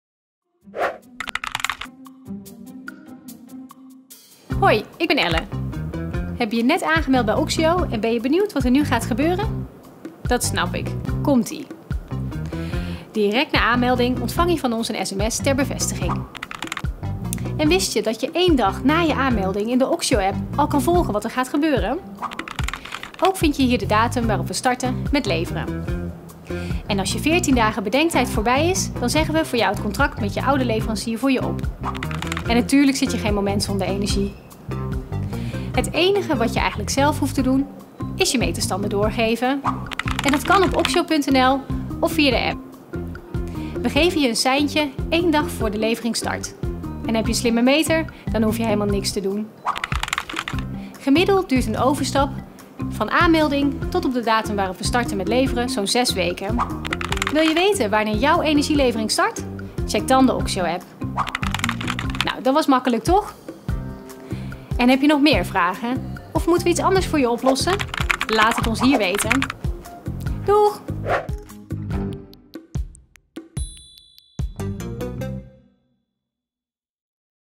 Overstappen naar Oxxio? Al je vragen Opgelost!
Wil jij weten wat er gebeurt als je overstapt naar Oxxio? Ellen legt je uit welke stappen je doorloopt en hoe lang het duurt voordat we je energie leveren. Ze vertelt je ook hoe handig de Oxxio App bij een overstap is.